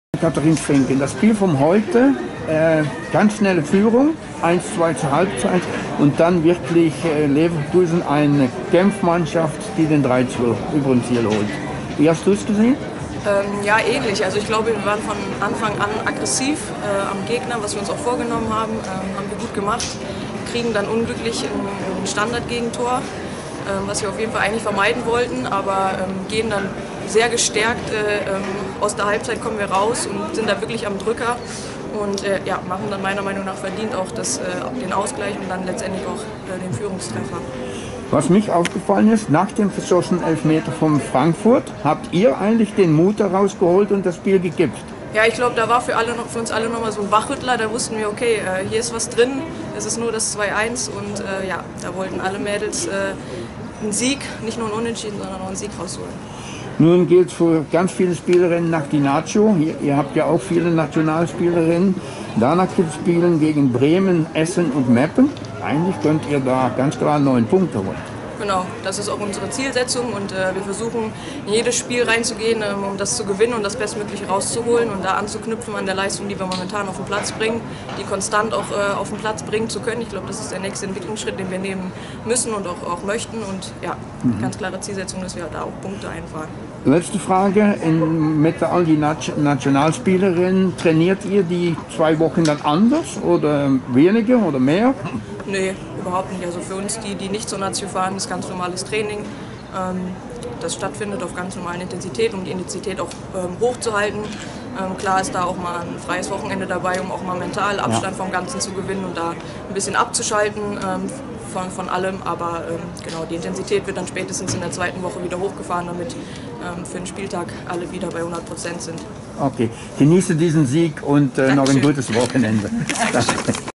Interview mit Bayer04 Leverkusen Spielerin Ann Katherin Vinken nach dem 3-2 Sieg gegen E. Frankfurt
Interview mit Bayer04 Leverkusen Frauen Spielerin Ann-Katherin Vinken nach dem Heimspiel gegen Eintracht Frankfurt Frauen im Flyeralarm Frauen Bundesliga Saison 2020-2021 Runde 16 Spiel. Die Begegnung fand statt im Ulrich-Haberland-Stadion von Leverkusen am Freitag 26 Marz 2021in ging aus auf einen 3-2 Sieg für die Werkself.